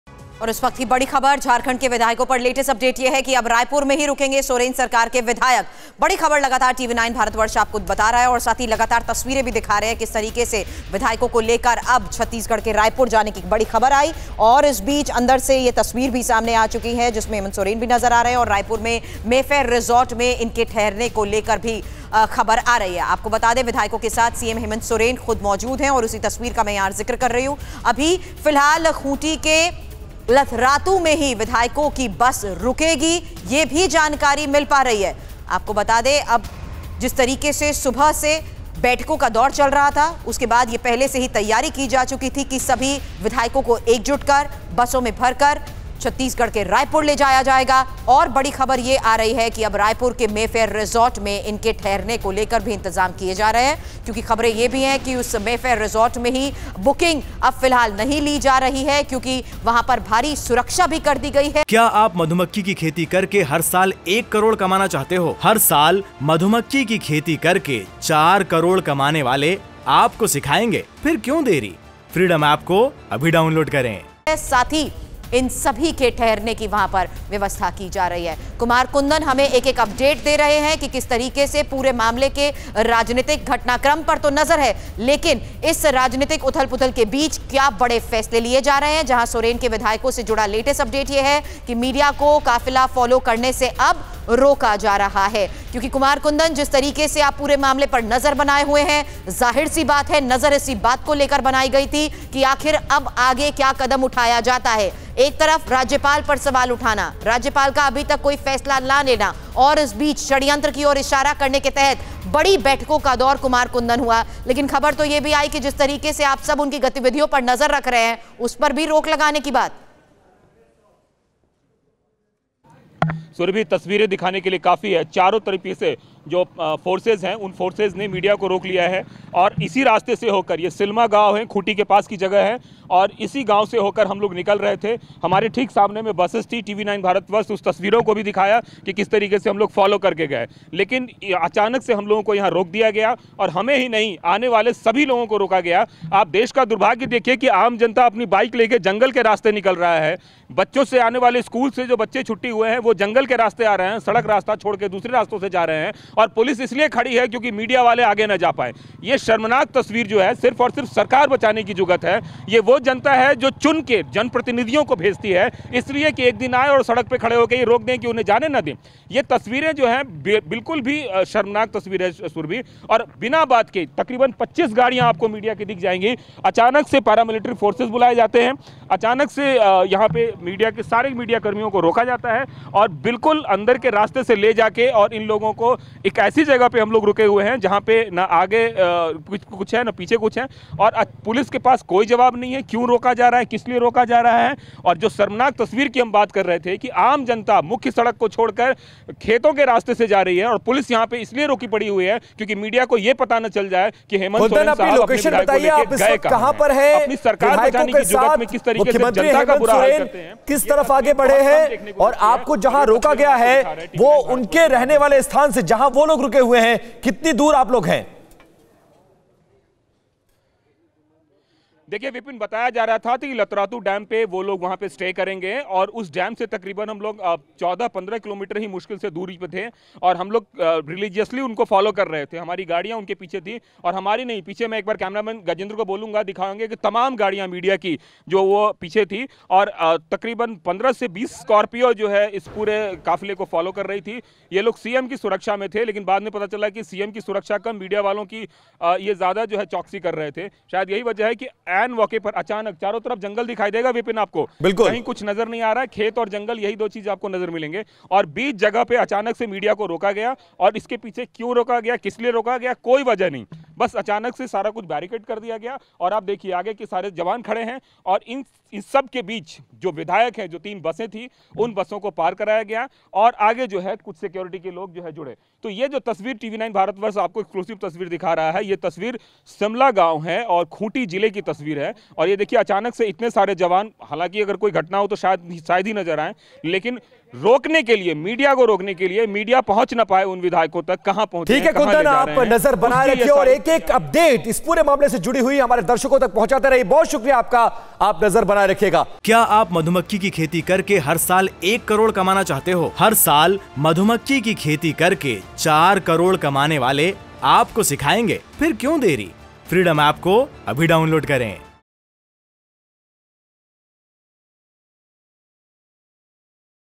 Raipur में रुकेंगे Hemant Soren सरकार के विधायक, अभी Khunti के लतरातू में रुकेगी विधायकों की बस
झारखंड के विधायकों पर लेटेस्ट अपडेट. रायपुर में रुकेंगे सोरेन सरकार के विधायक. रायपुर के मे-फेयर रिसॉर्ट में ठहर सकते हैं विधायक. विधायकों के साथ सीएम हेमंत सोरेन भी मौजूद. अभी खूंटी के लतरातू में रुकेगी विधायकों की बस
Latest updates on Jharkhand MLAs. MLAs of Soren government will stay in Raipur. MLAs can stay at May-Fair Resort in Raipur. CM Hemant Soren was also present along with the MLAs. Now the bus of MLAs will stop at Latratu in Khunti